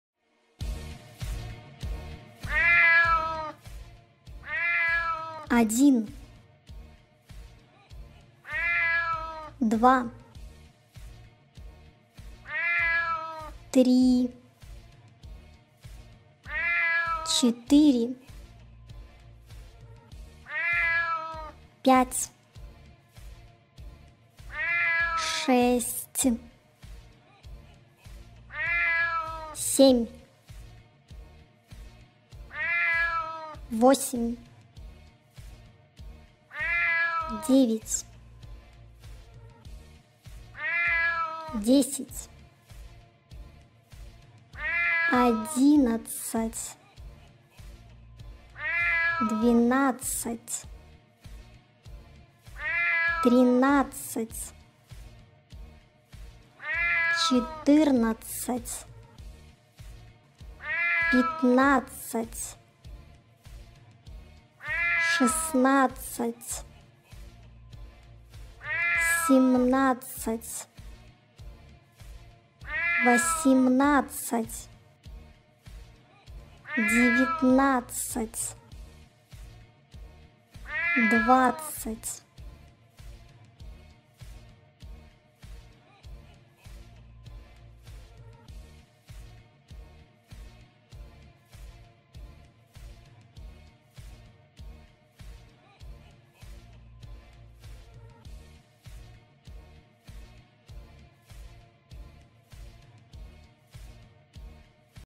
Halloween Black Cat Teaching to Count 1 to 20 in Russian Language and Numbers Video for Kids
Watch Halloween Black Cats and witches while you learn. You've found the best educational cartoon video!  Learn about alphabets, math, counting, numbers, colors, music, and more with these educational cartoon parade for kids!

Some of the images in this video were designed by vectorpocket / Freepik.